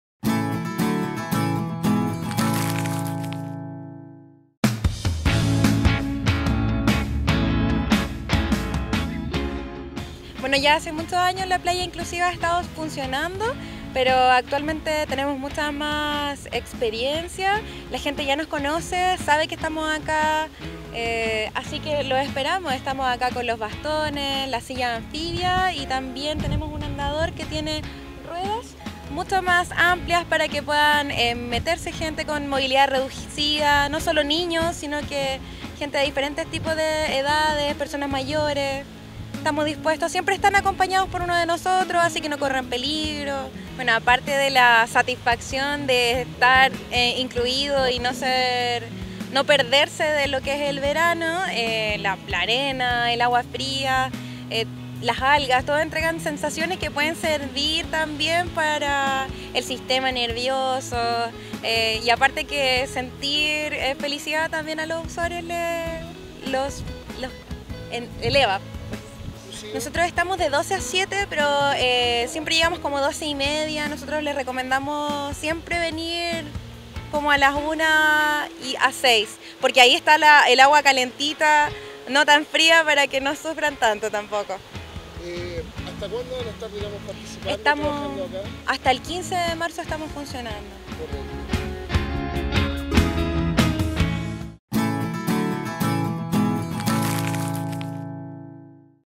Playa Inclusiva Verano 2023
Este martes comenzó a funcionar la Playa Inclusiva del municipio, que cuenta con un amplio equipamiento y grupo de profesionales para que las personas en situación de discapacidad o con movilidad reducida puedan disfrutar de este verano en la comuna.

El servicio es completamente gratuito y pueden acceder a el de martes a domingo entre las 12:30 y 18:00 horas en la Playa Los Pescadores en el centro de Quintero.